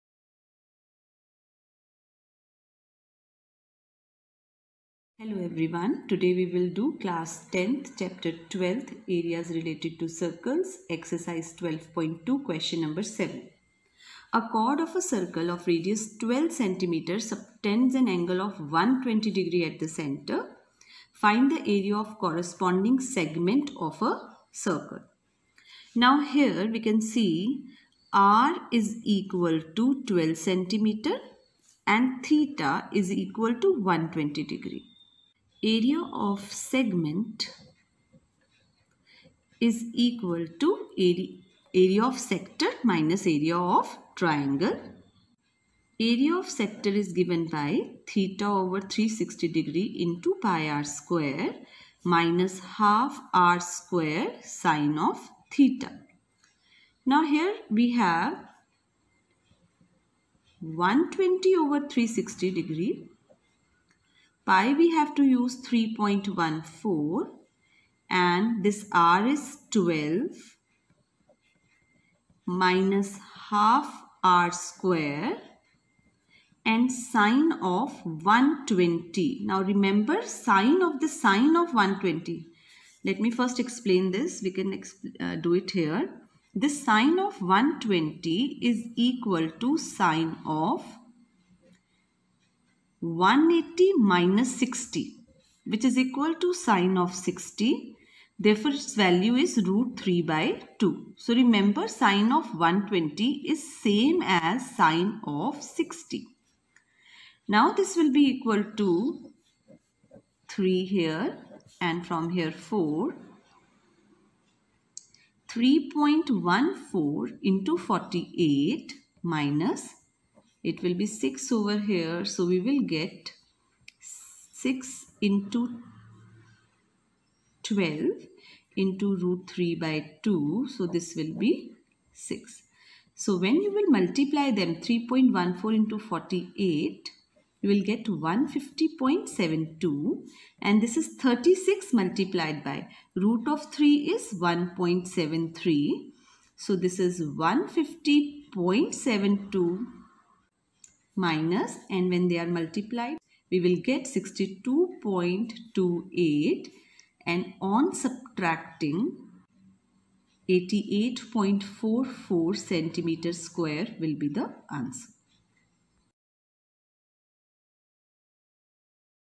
CLASS 10 | AREAS RELATED TO CIRCLES | EX. 12.2 SUM NO. 7
A chord of a circle of radius 12 cm subtends an angle of 120 degrees at the center. Find the area of the corresponding segment of the circle.